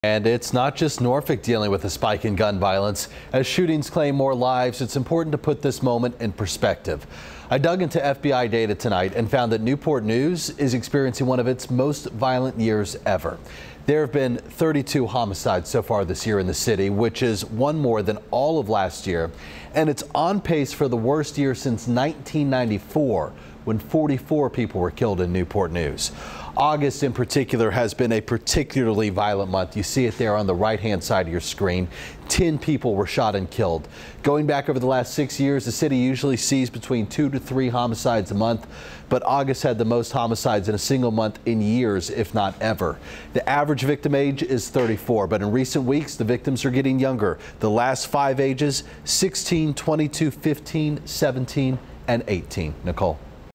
10 homicides in August push Newport News violence to near-record levels
The victims are getting younger, with four teens murdered in two weeks.